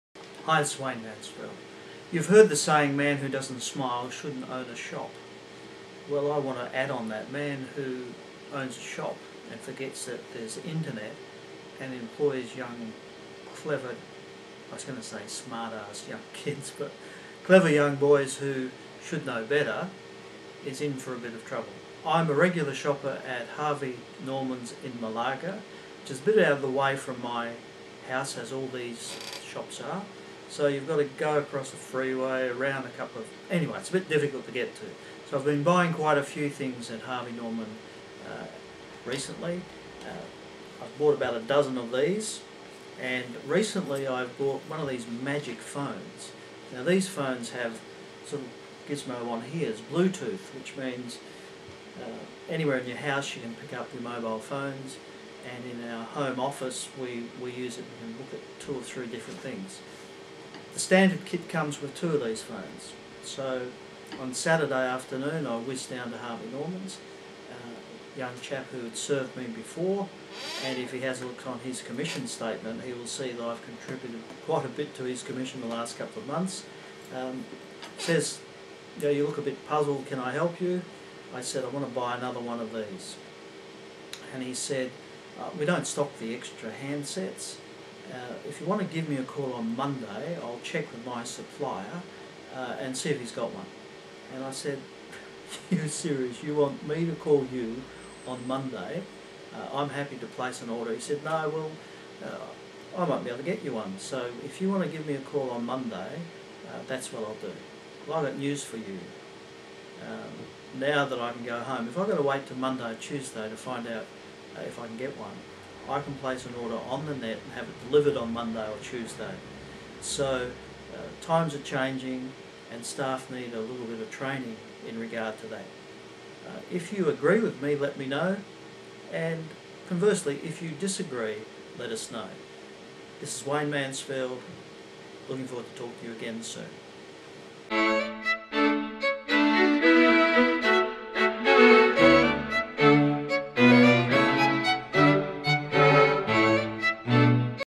Wayne Mansfield - Customer Service at Harvey Norman
Wayne Mansfield talks to you about his experiance in Harvey Norman and customer service.